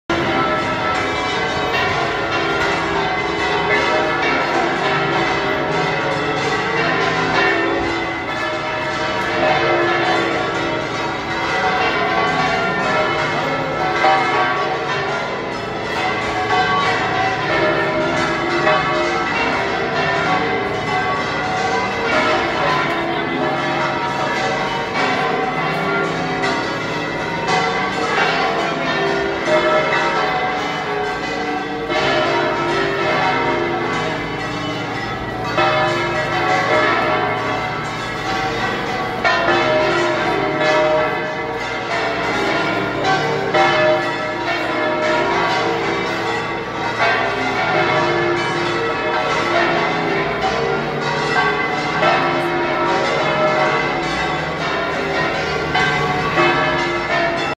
The bells of La Giralda in Seville, Spain
60 seconds of the bells of La Giralda pealing at 8pm on Sunday 14 August 2022. They're loud! I recorded this on a visit to the city. The complete pealing lasted for more than 4 minutes.

La Giralda is the bell tower of Seville Cathedral in Seville, Spain. It has a distinct and storied history from its origin as the minaret for the Great Mosque of Seville in al-Andalus, Moorish Spain, during the reign of the Almohad dynasty in the 12th century CE, with a Renaissance-style belfry added by the Catholics after the expulsion of the Muslims from the area in the 13th century.